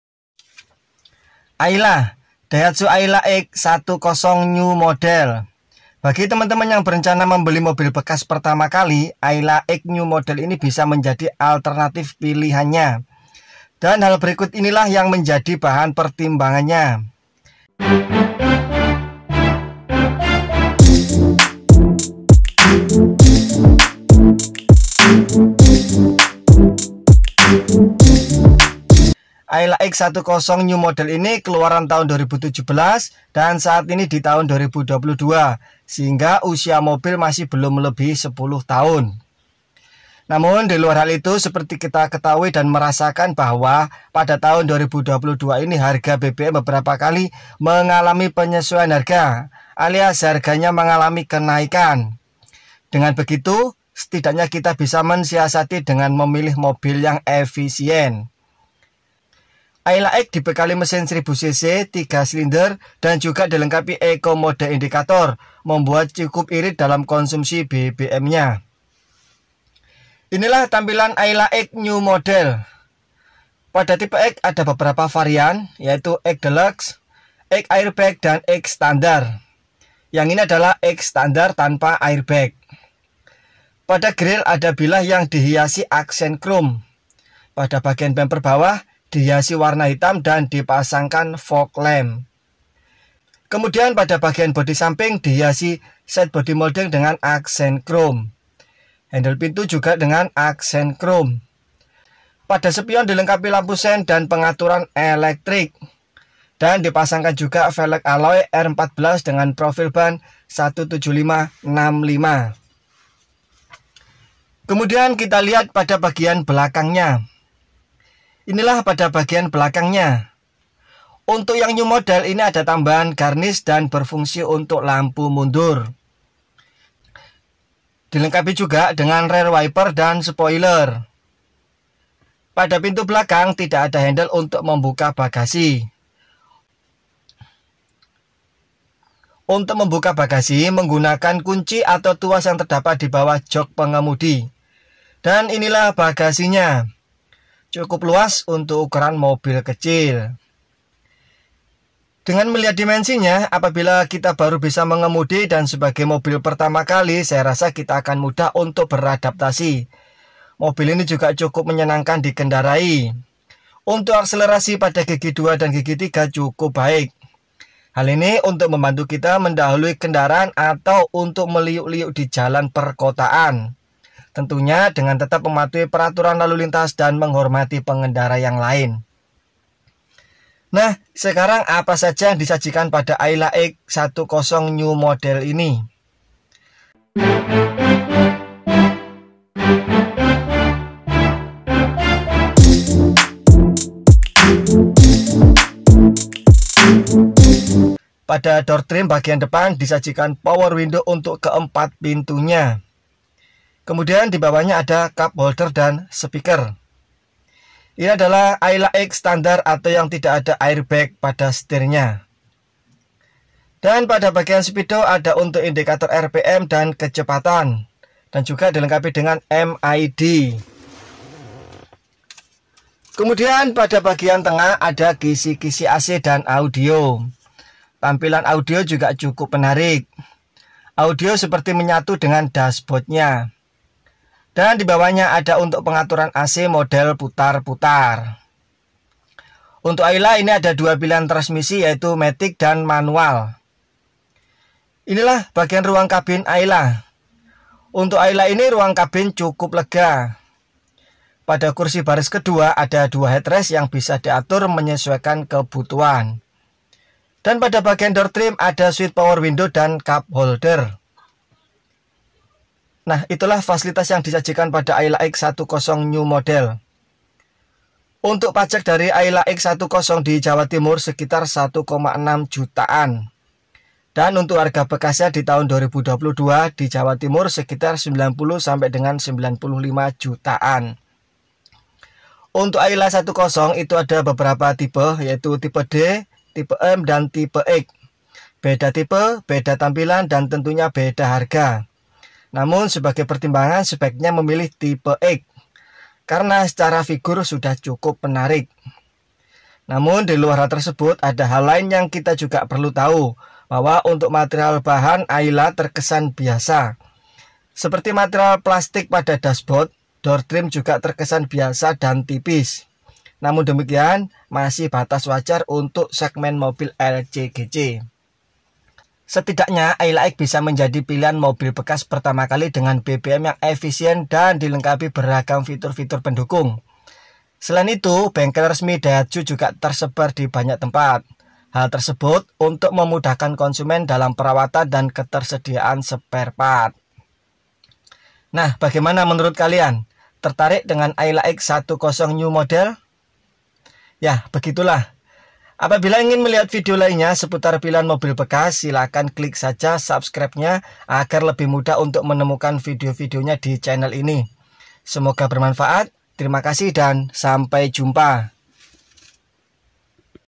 PILIHAN MOBIL BEKAS PERTAMA KALI | AYLA X 1.0
Daihatsu Ayla x 1.0 ini hadir pada tahun 2017,desain baru dan perubahan desain tampak berbeda dengan yang sebelumnya,penyegaran tampilan depan dan bagian belakang.pada tahun 2017 silam Daihatsu meluncurkan 2 jenis Ayla dengan pilihan mesin 1000cc dan 1200cc,secara tampilan berbeda dari keduanya dan mudah untuk mengetahuinya.untuk Ayla 1000cc ini ada beberapa type dari type D D+,M dan X.
Ayla ada 2 piliham 2jenis transmisi yaitu matic sqn manial.
Dari video tersebuy berdasarkan apa yang saya ketahui,apabilq ada kesalahan dam kekurangan silahkan koreksi dam tulis di kolom koentar.Terimakasih